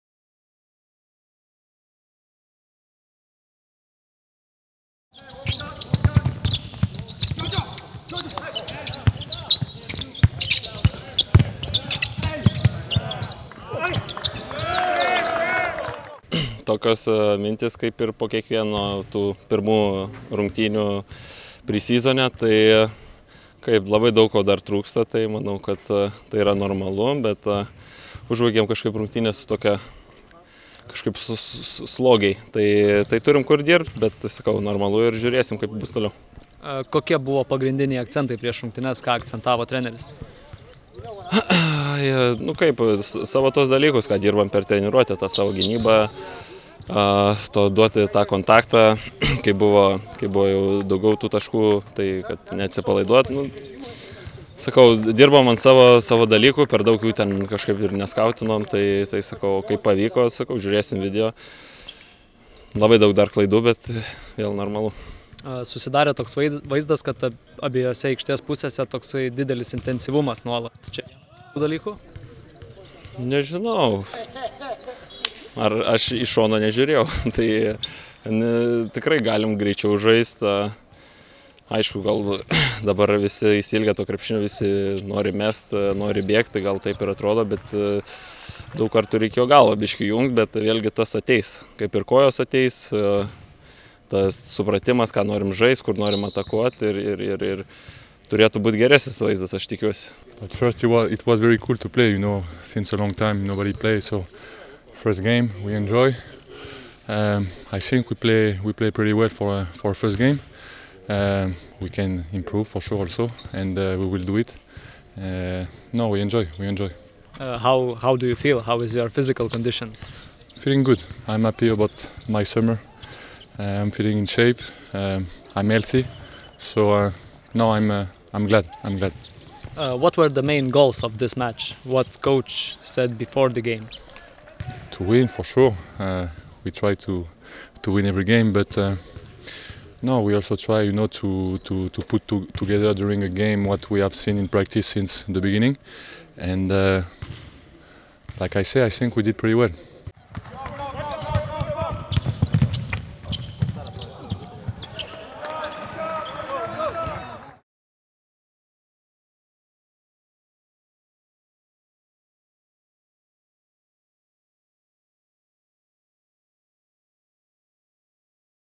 Grigonis' and Lauvergne's takeaways on first pre-season win
Marius Grigonis and Joffrey Lauvergne shared their thoughts after the victory in Zalgiris' preseason opener against VEF Riga.